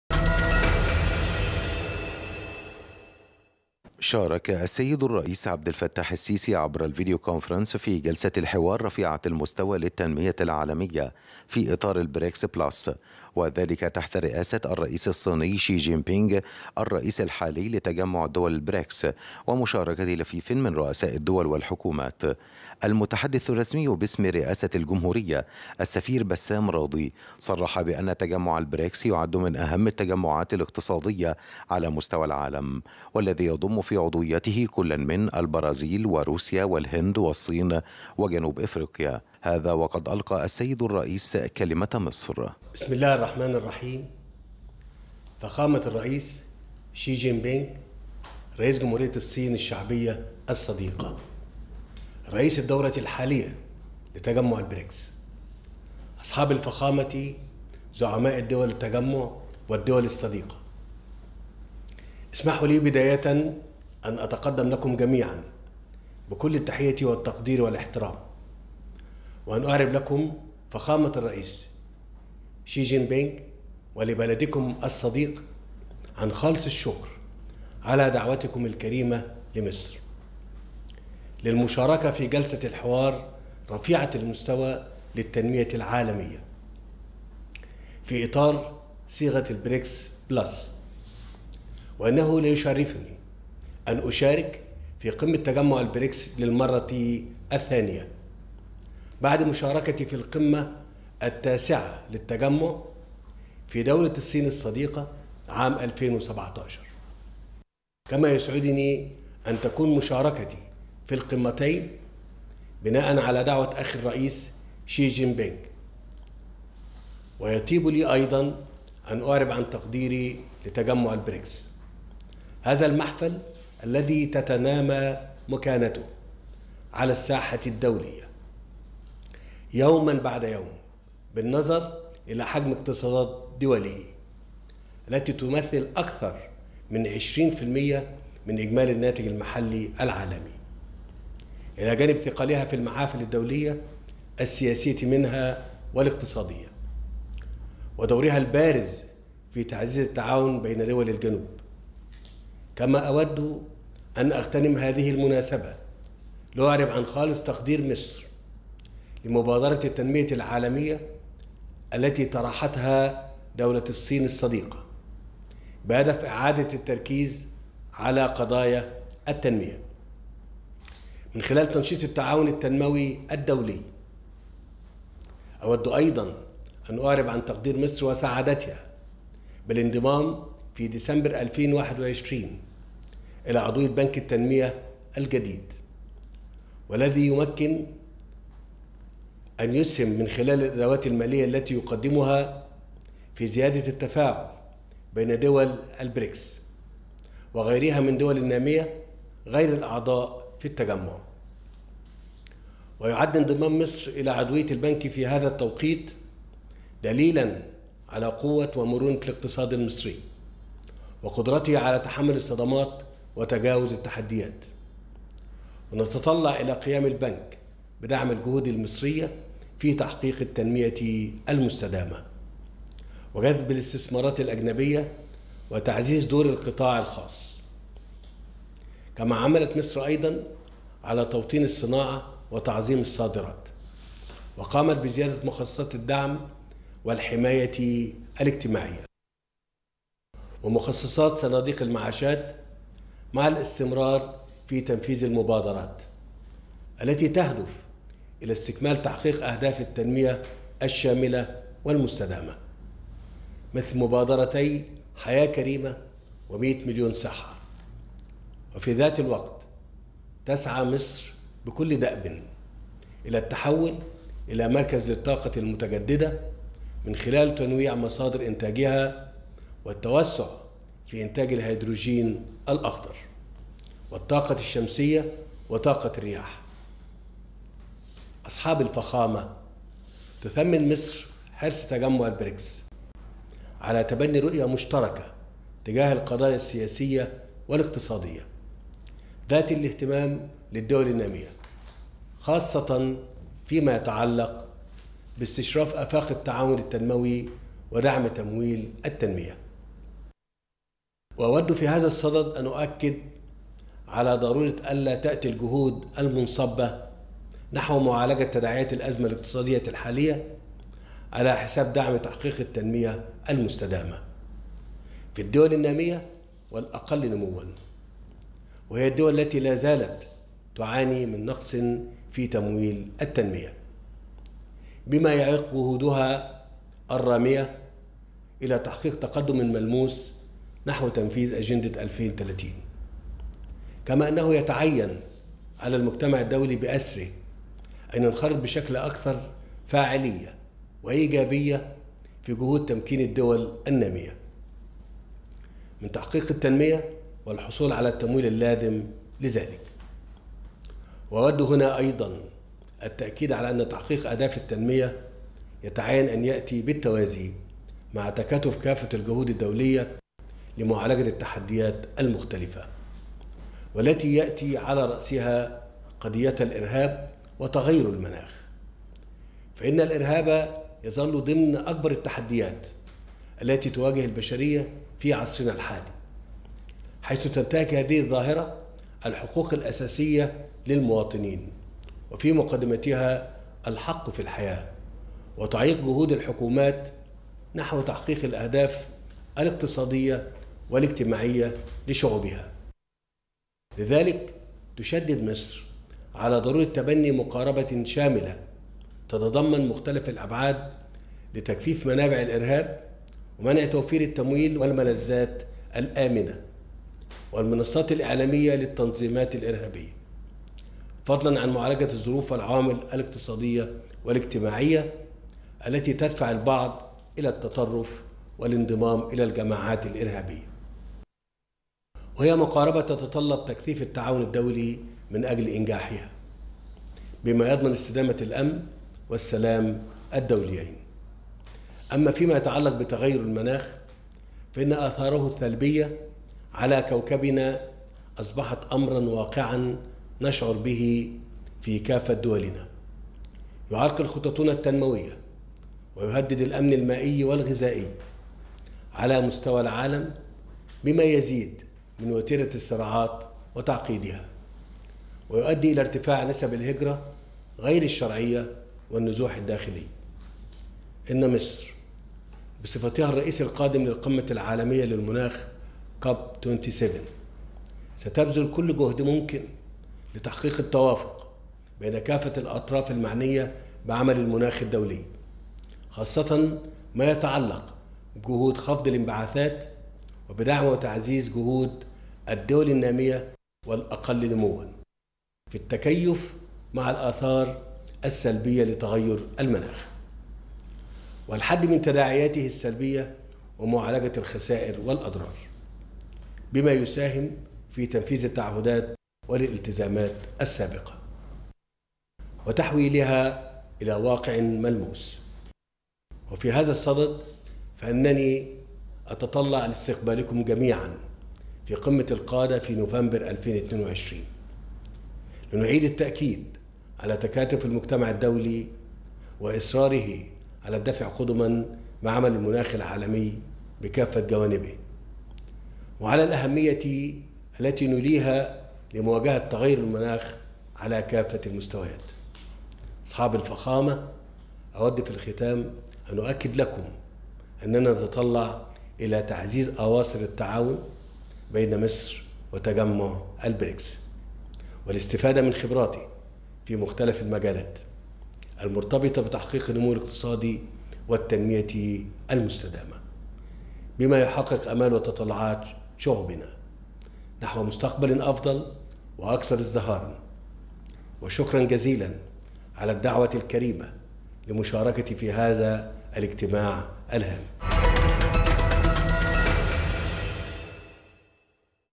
تغطية خاصة | كلمة الرئيس السيسي بجلسة الحوار رفيعة المستوى للتنمية العالمية في إطار البريكس بلس
تغطية خاصة | كلمة الرئيس السيسي بجلسة الحوار رفيعة المستوى للتنمية العالمية في إطار البريكس بلس عبر الفيديو كونفرانس